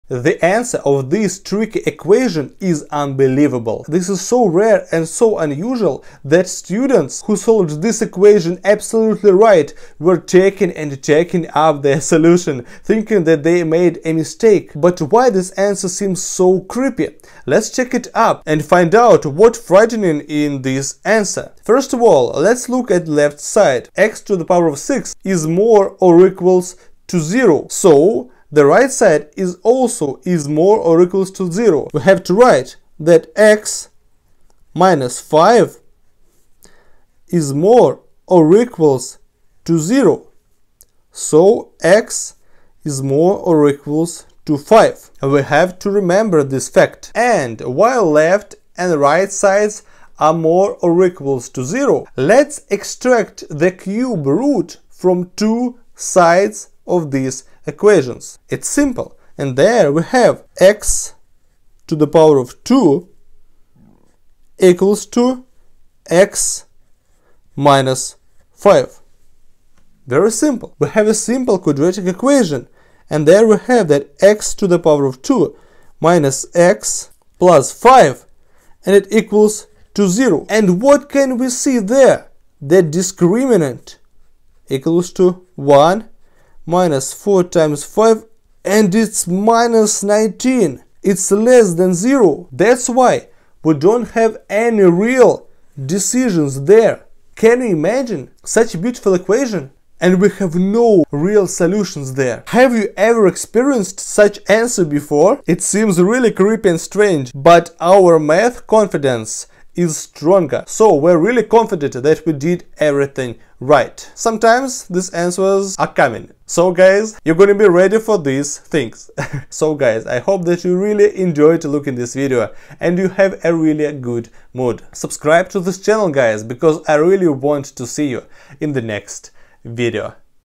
Only 1 of 1000 Solved This! Olympiad Mathematics.
Olympiad mathematics. Nice algebra equation. Equation solving. You should use this trick.